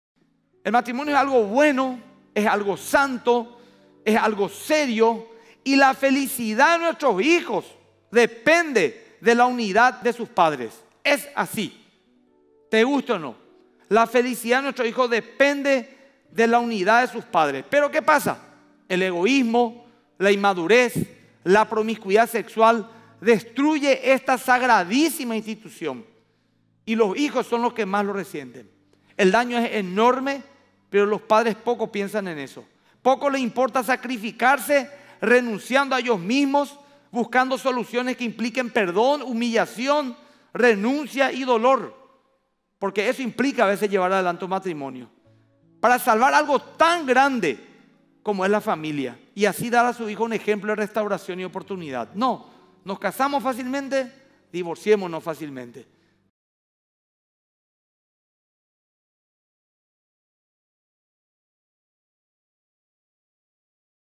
MDI-034 Minuto de Impacto MQV - El matrimonio y la felicidad de los hijos
El matrimonio es santo, bueno, serio con una solidez que lo debe hacer irrompible.
Esto se ha perdido con el tiempo, los divorcios abundan y en muchas ocasiones su causa es la inmadurez, el egoísmo y la promiscuidad sexual. Se ha olvidado que la felicidad de los Hijos depende de la unidad de los padres.
No se trata de mantener una apariencia de “Hogar”, sino que a pesar de las imperfecciones con amor se dé un ejemplo de perdón, amor y humillación en dónde ambos cónyuges cedan sus intereses personales, por un bien mayor… “LA FAMILIA”

Fragmento de la prédica “Herederos de bendición” del pastor Emilio Agüero Esgaib a ser emitida este domingo 13 de octubre a las 10:00 de la mañana por la RPC (Canal 13).¡Compártelo!